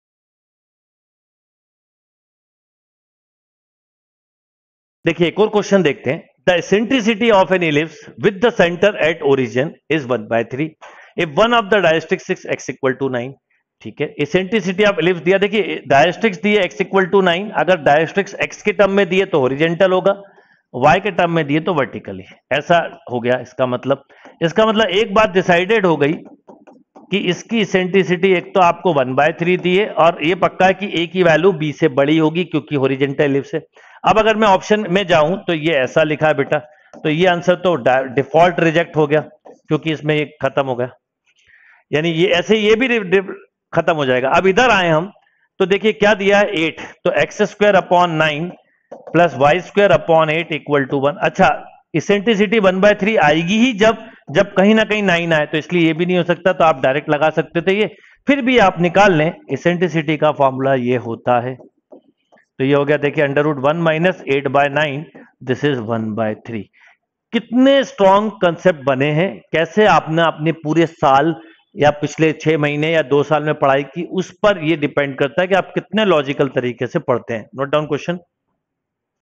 The eccentricity of an ellipse, with its center at the origin is 1/3. | NIMCET 2022 |Impetus Gurukul
The eccentricity of an ellipse, with its center at the origin is 1/3. If one of the directrices is x=9, then the equation of ellipse is:
(a) 9x^2+8y^2=72 (b) 8x^2+9y^2=72
Ans: b